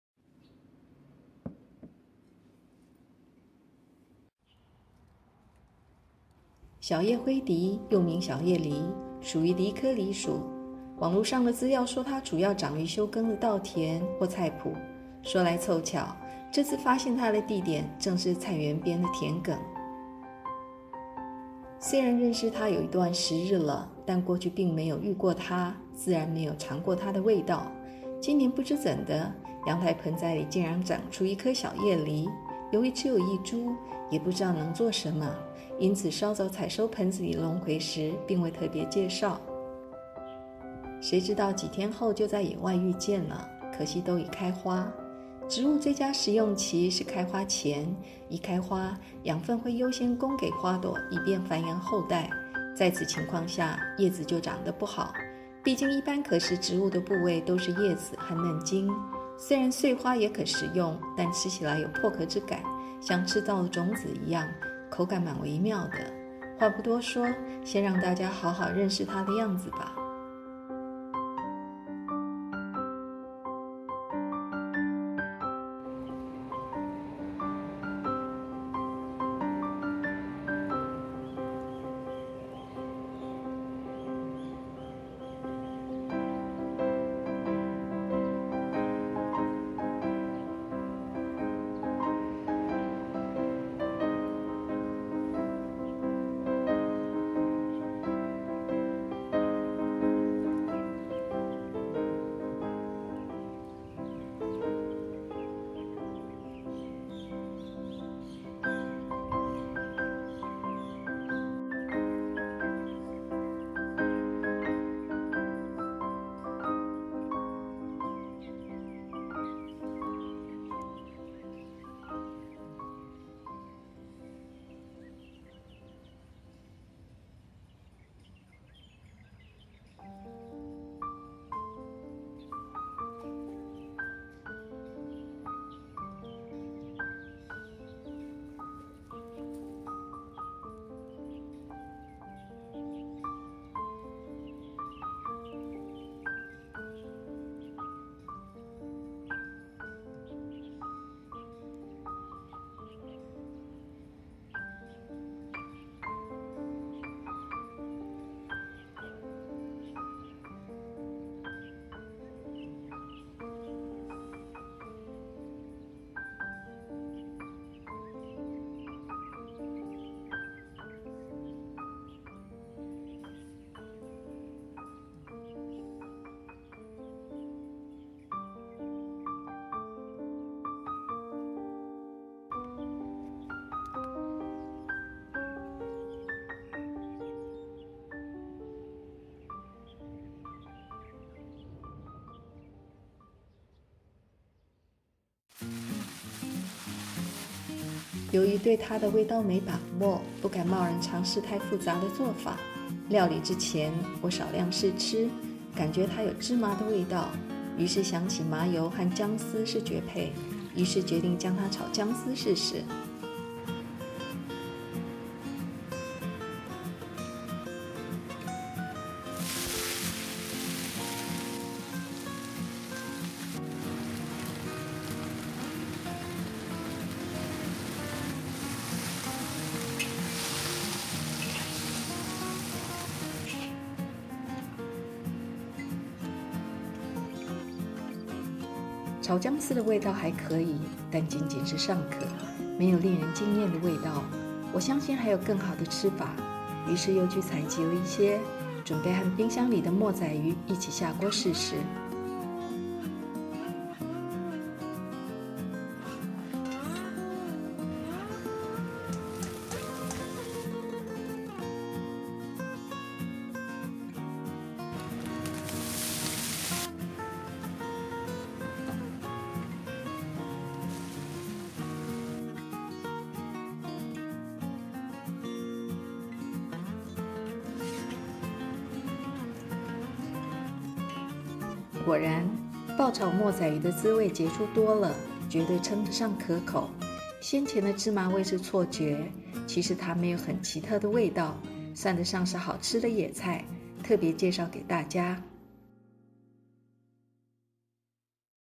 告訴你小葉藜的滋味，不要錯過！Small Goosefoot（Chenopodium serotinum）
小葉灰藋又名小葉藜，是好吃野菜